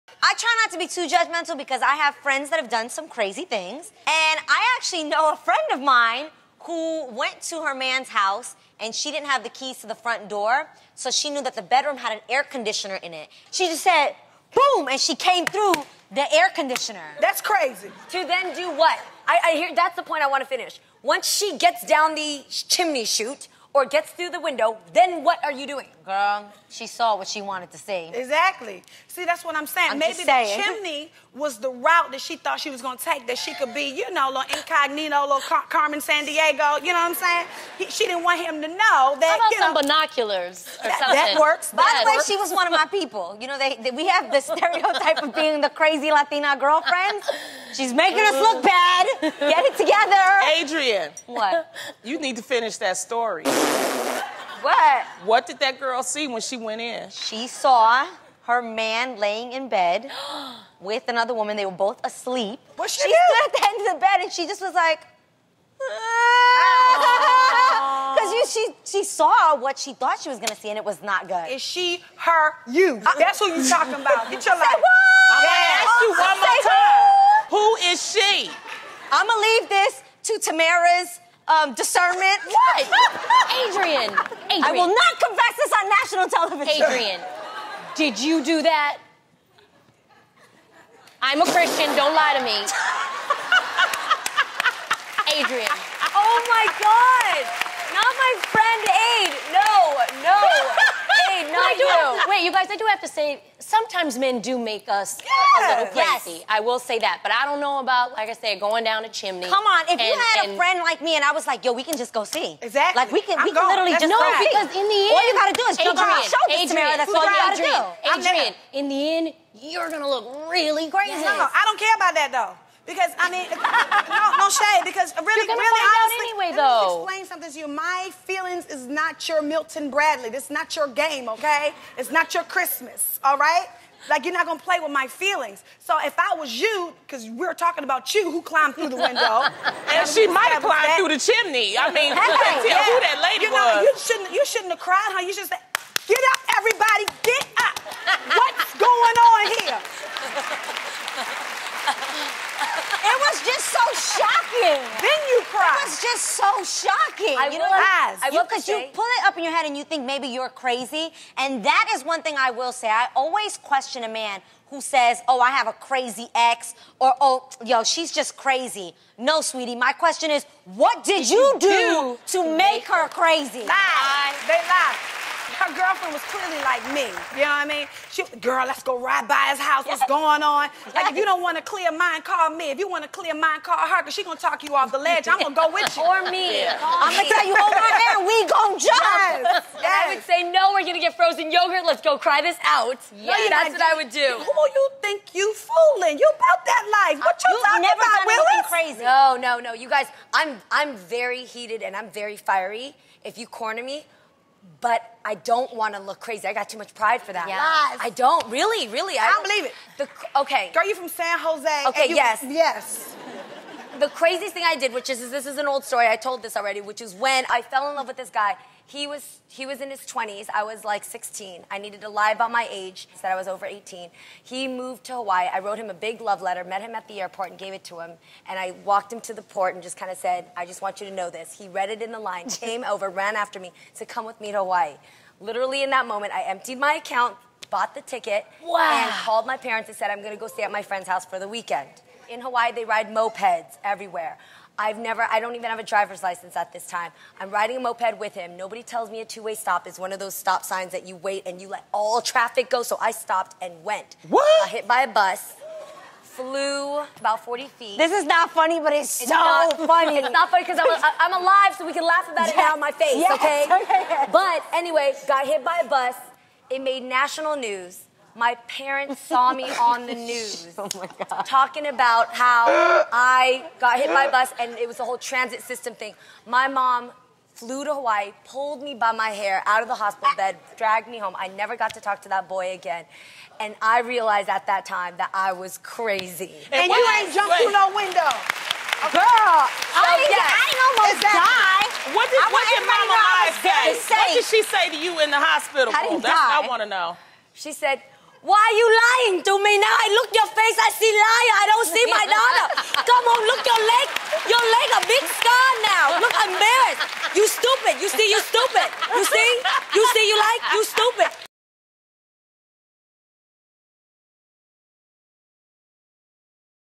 Crazy Things We Do for Love
Let’s be real — we’ve all been crazy in love once or twice. During this Girl Chat, Adrienne and Jeannie open up about their wildest romance moments. Let’s just say there’s no limit to their love!